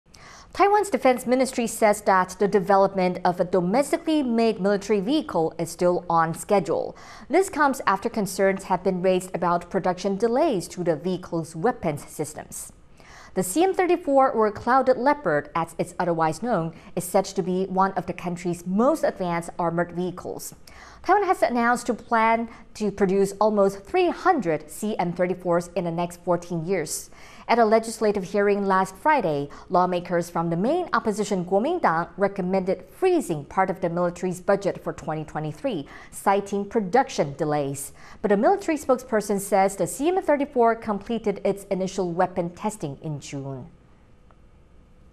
Taiwan's Military Says Clouded Leopard Armoured Vehicle on Schedule | TaiwanPlus News
📹Reporters: Asnaya Chou/Jaime Ocon

Taiwan’s defense ministry says that the development of a domestically made military vehicle is still on schedule. This comes after concerns have been raised about production delays over the vehicle’s weapons system.